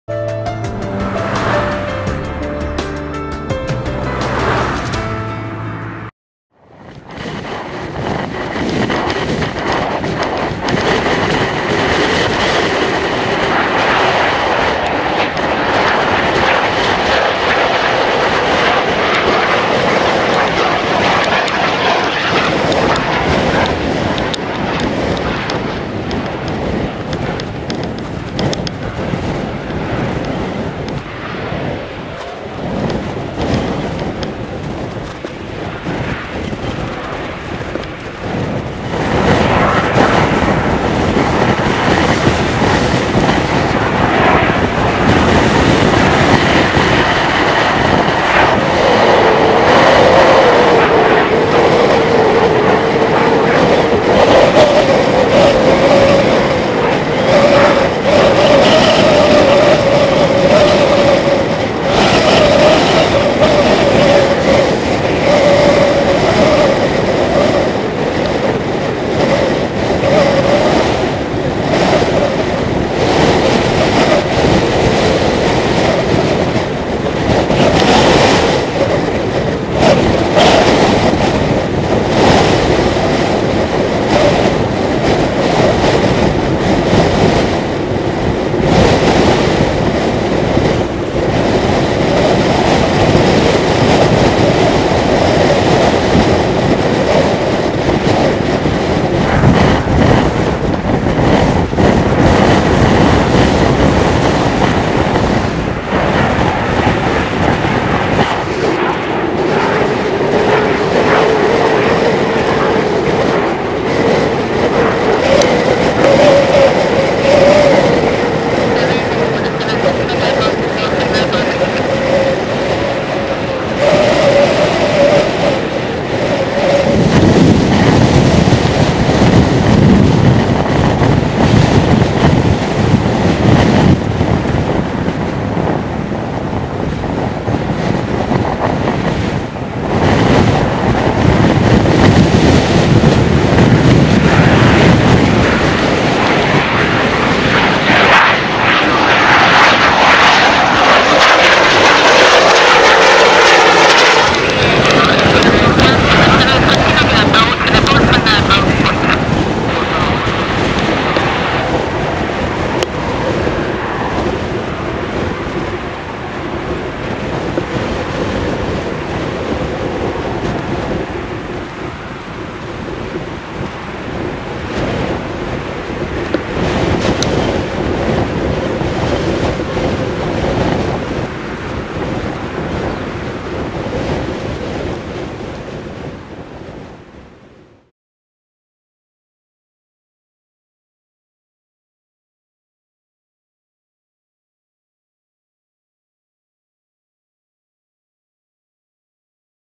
Pilatus PC-21E-27 "Mirlo" Error o Acierto?
Pilatus PC-21E-27 "Mirlo" operando en la base aerea de Zaragoza  (ZAZ)
Es sin duda importante la controversia generada en torno a la decisión de la elección de este modelo de avión turbohélice como entrenador para los futuros pilotos del ejército del Aire español en lugar de un modelo a turbina.

Pilatus PC-21E-27 "Mirlo" operating at the Zaragoza air base (ZAZ)
The controversy generated around the decision to choose this model of turboprop aircraft as a trainer for future pilots of the Spanish Air Force instead of a jet model is undoubtedly important.